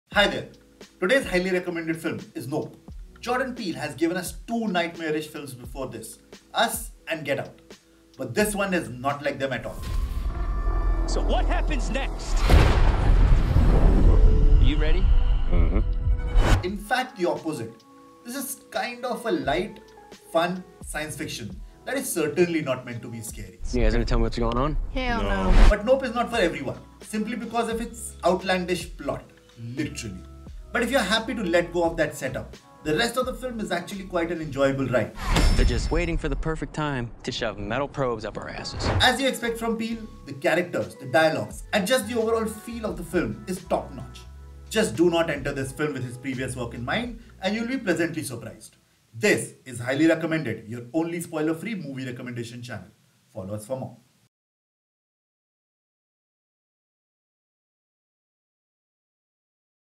Jordan Peele's NOPE - A misunderstood GEM or A failed attempt to venture out of his comfort zone?
If you want Peele's stamp on a film, Nope is not for you! But that doesn't mean it isn't fun! Watch the review for more.